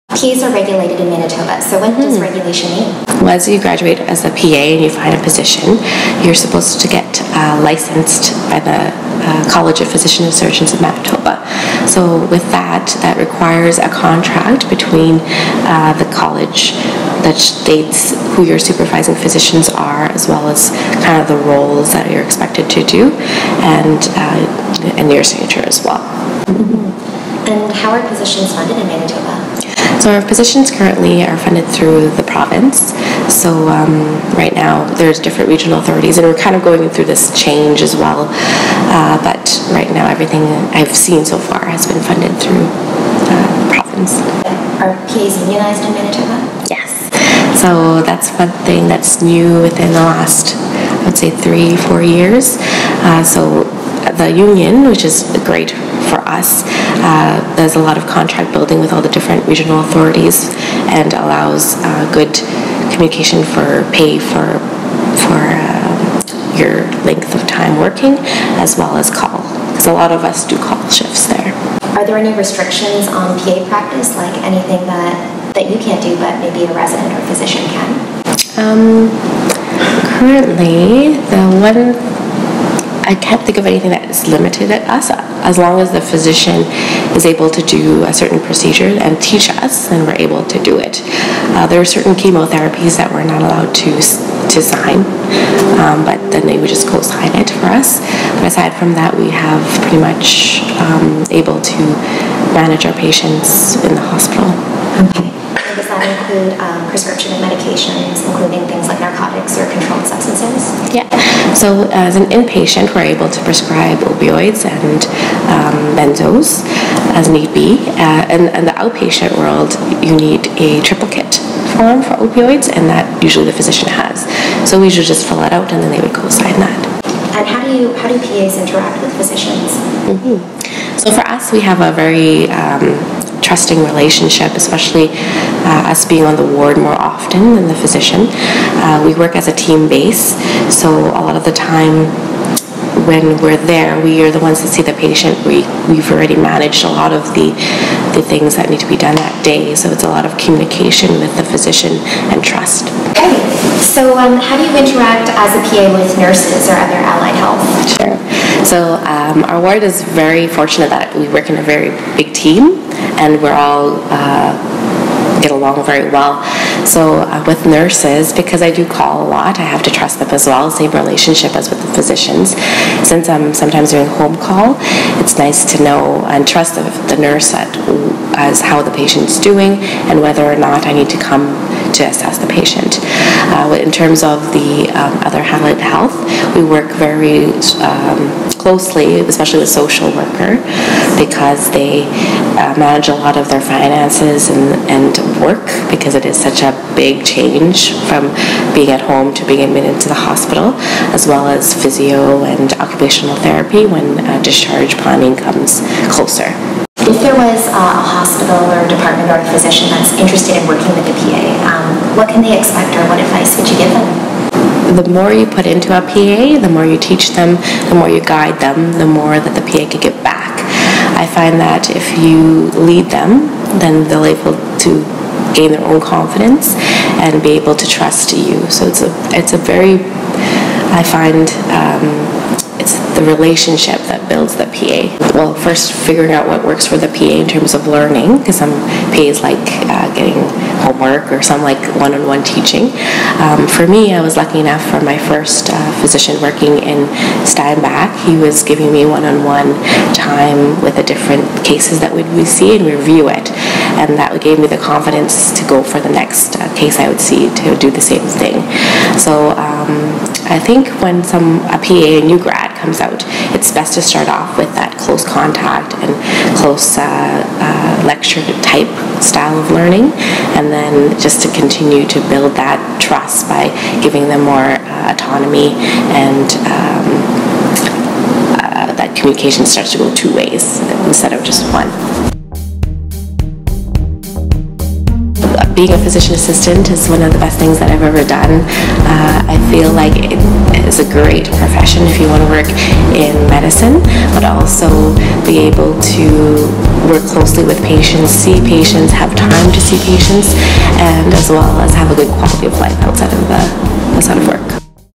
PA Practice in Manitoba - Scope, Regulation and Funding
Eden is a Manitoba PA graduate with experience working in rural Manitoba as well as Winnipeg, Manitoba.

Here she explains what regulation means to an individual practicing PA, scope of practice, and performing prescriptions.

A quote when I asked Eden, "What do you recommend to hospitals or PA employers if they are thinking about adding a PA to their practice?"

The more you put into a PA and the more you teach them, the more you guide them, then the more that the PA can give back.

I find that if you lead them, then they’ll be able to gain confidence and be able to trust you.

It’s the relationship that helps build the PA.

First it’s figuring out what works for the PA in terms of learning because some PAs like getting homework, or some like one-on-one teaching. For me, I was lucky enough for my first position, working in Steinbach, the physician was giving me one-on-one with the different cases we would see and review it, and that would give me the confidence to go for the next case I would see and do the same thing.

I think that when a new grad comes out, its best to start with off with that close contact, lecture-type style of learning and then just continue to build that trust by giving them more autonomy"

❐ RESOURCES MENTIONED ❏

❐ JOIN US ON OUR ON OUR FREE FB GROUP ❏